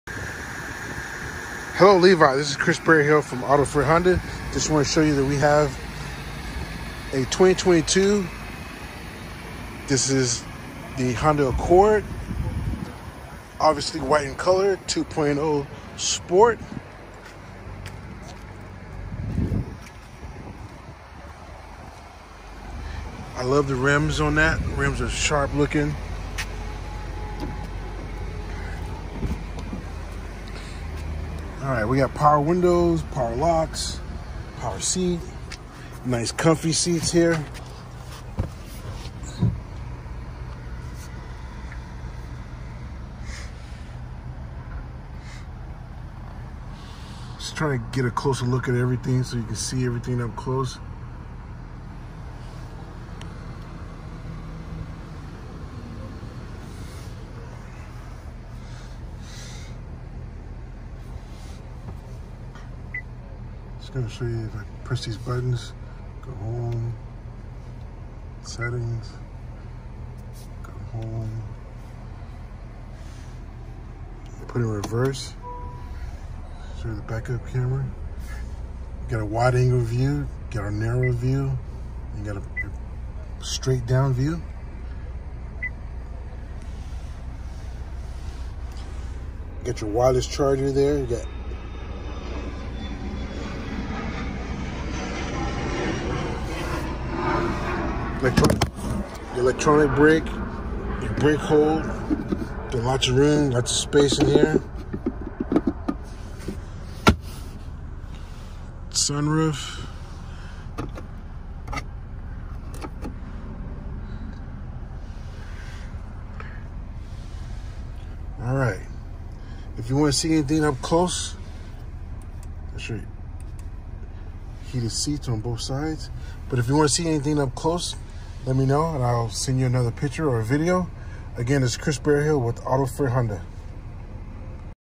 Levi, Here’s a 2022 Honda Accord two. Oh sport
Chris Berryhill here from AutoFair Honda in Manchester, NH with a quick video that you’re interested in of the 2022 Honda Accord 2.0 sport… If you have any questions please give me a call ￼